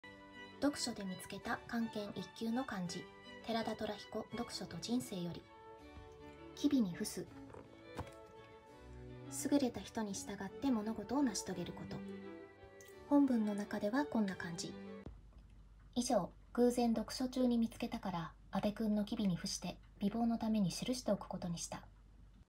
読書で見つけた漢検1級の漢字 寺田寅彦「読書と人生」
角川ソフィア文庫
寺田寅彦「読書と人生」より